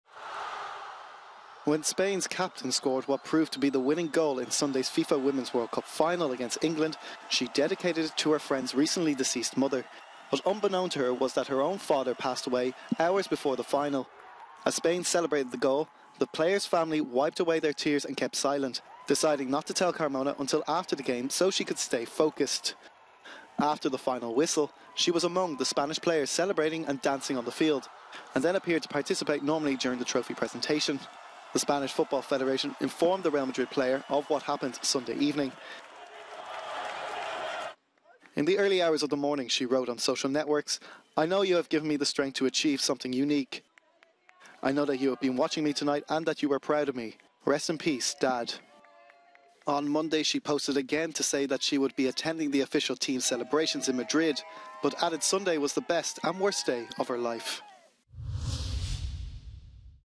Olga Carmona scored in Spain’s 1-0 Women’s World Cup win. Then she learned her father had died
After the final whistle, she was among the Spanish players celebrating and dancing on the field and appeared to participate normally during the trophy presentation.